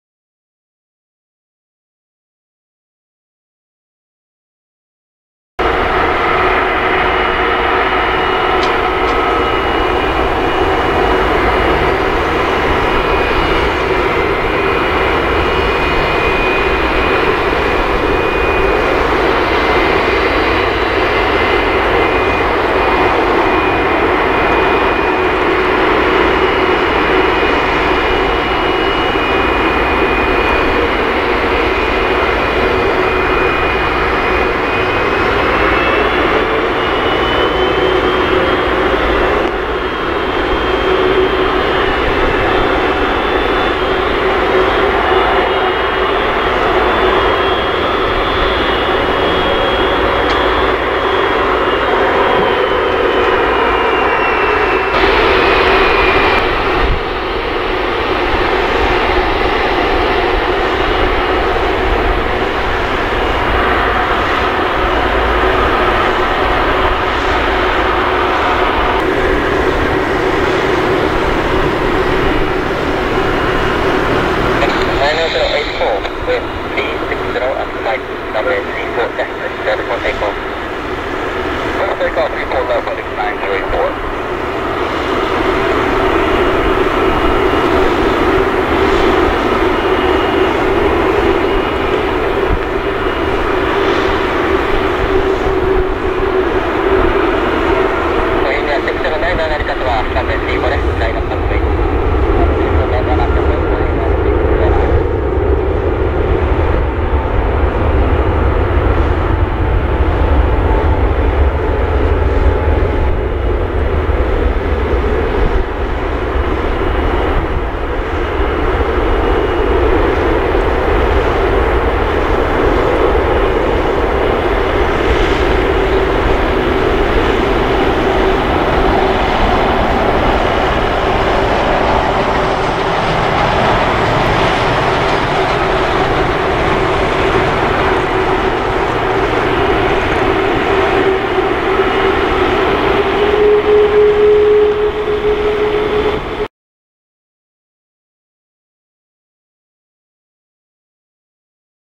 ✈✈Twilight of Narita 超珍しいBoeing 757-236(SF)フェデックス・エクスプレス (FedEx Express) N949FD Narita RWY34L成田空港
フェデックス・エクスプレス (FedEx Express)
Boeing 757-236(SF)
N949FD